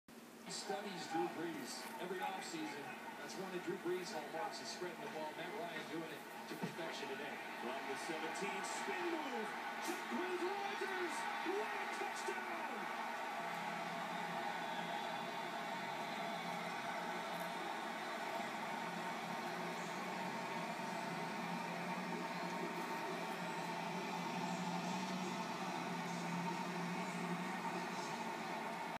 Jacquizz Rodgers spin move touchdown
A handoff to Jacquizz Rodgers that leads to a touchdown